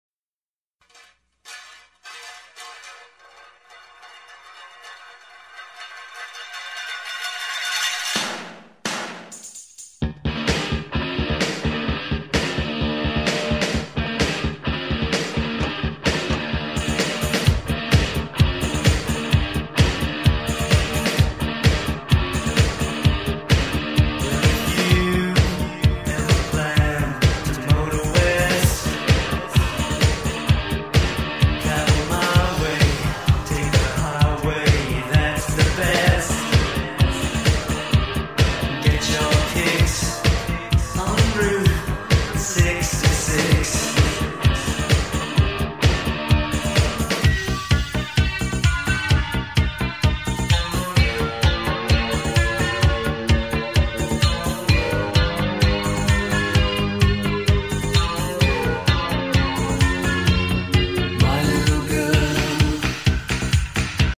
August Storm in the Ozarks
Fifteen minutes of event compressed to one minute of video.